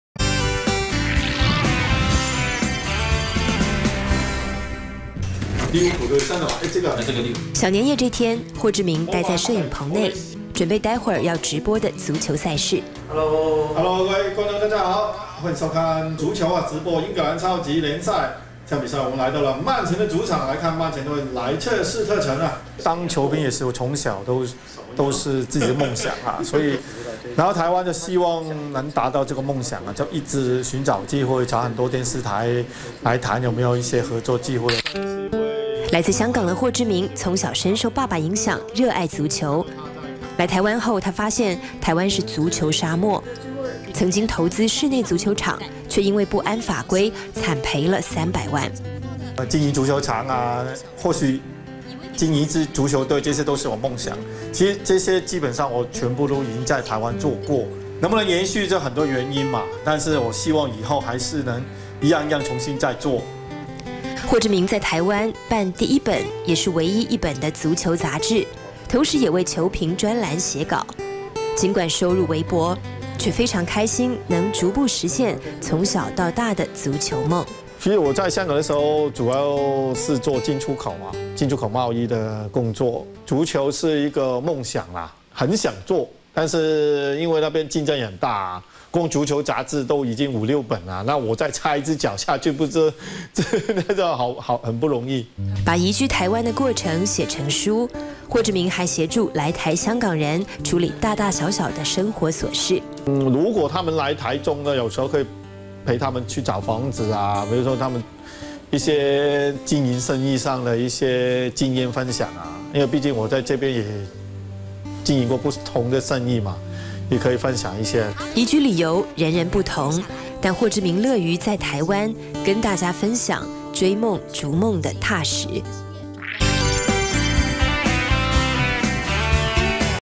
20160528 香港球評的足球夢
霍志明來自香港，1989年來台求學，2007年正式在台定居，來台12年，在台發行足球雜誌，投資室內球場，教小孩踢足球，他是標準的足球迷，愛看也愛踢足球，他希望能與大家分享在台灣生活的點點滴滴，並且讓大家更加認識台灣，因此之前在香港推出著作《移居台灣》，他很喜歡台灣，人、事、物都很美好，尤其是人，特別熱情，台灣是他現在唯一的家，未來他會朝著「盡一分力」的目標，讓台灣更了解足球的魅力，繼續在台築他的足球夢。